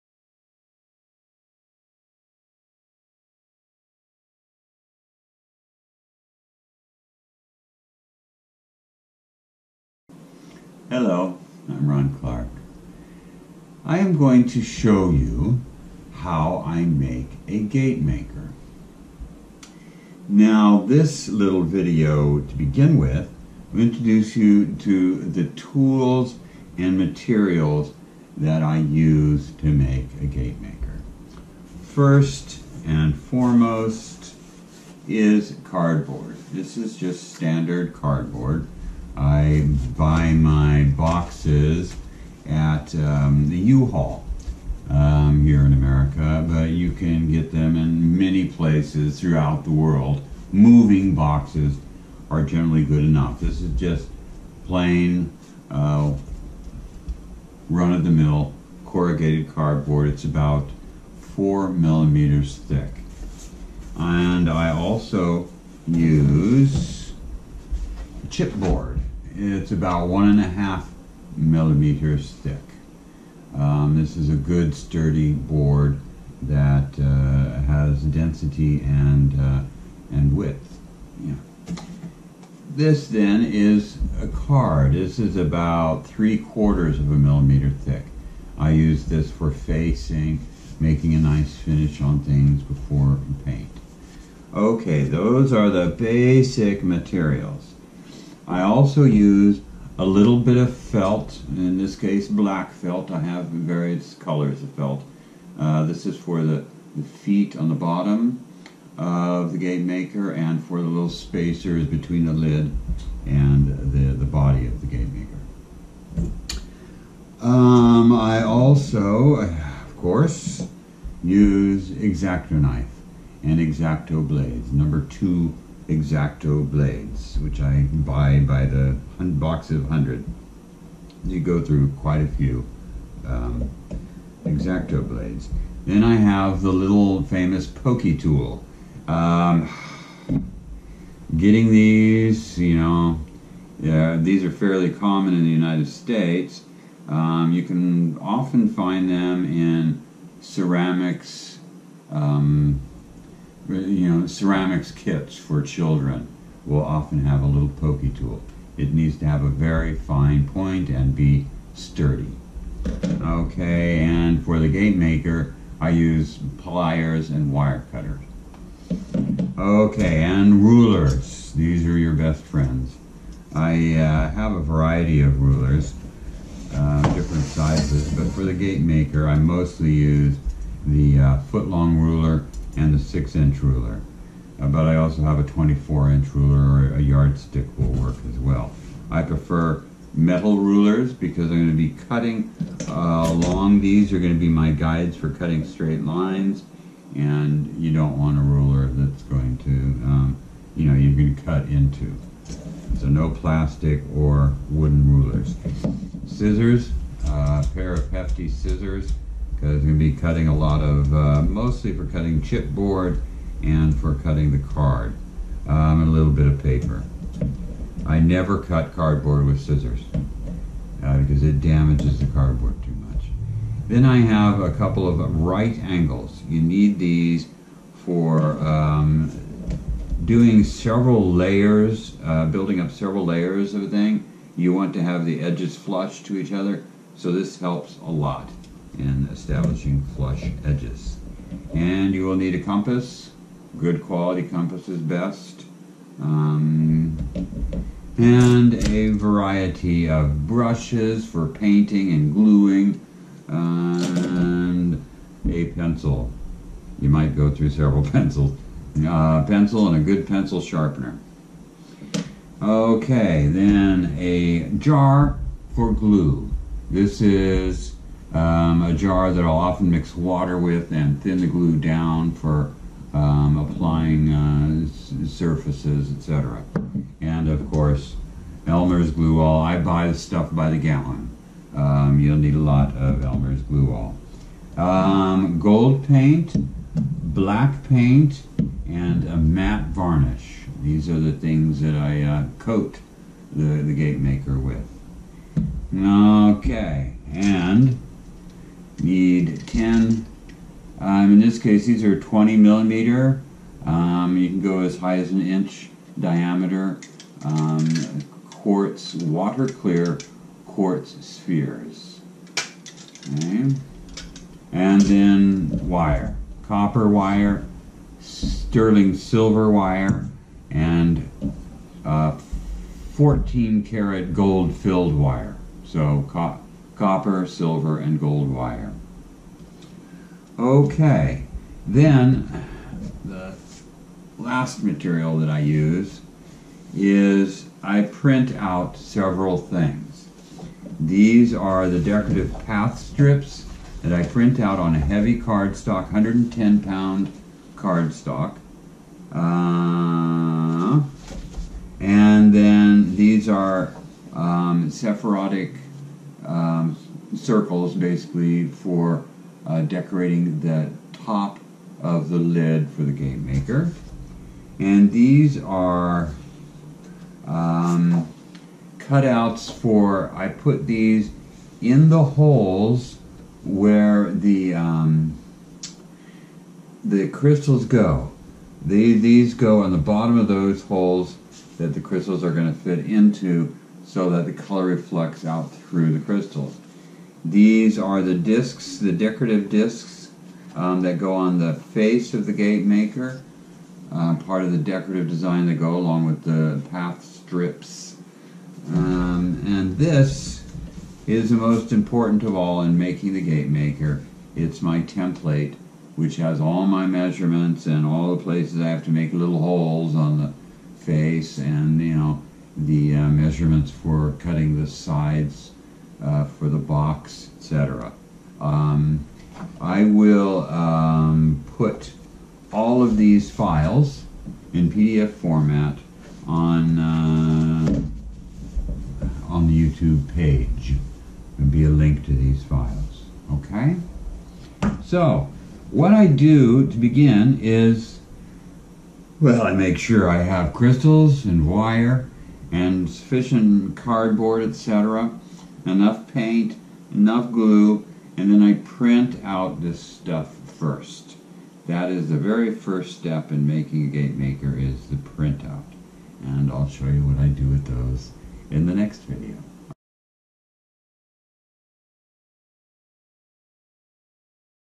How to make a Gate Maker -- Part 1: Introduction
A "how to" video by Rawn Clark!  Come join me as I construct a new Portable Gate Maker before your very eyes!  :)

In this first episode I talk about all the materials and tools I use in the building of a Gate Maker.  The print-outs mentioned in this video can be downloaded (as a .zip file) for your own use at this